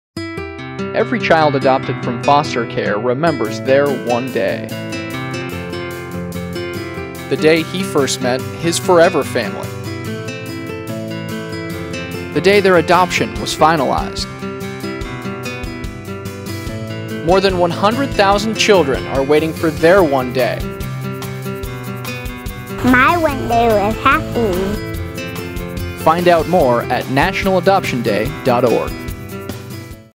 National Adoption Day 2013 PSA - My One Day
Every child adopted from foster care remembers their one day. The day he first met his forever family. The day their adoption was finalized. More than 100,000 children are waiting for their one day. Find out more at nationaladoptionday.org.

Video features National Adoption Day 2013 spokesperson Rachel Crow.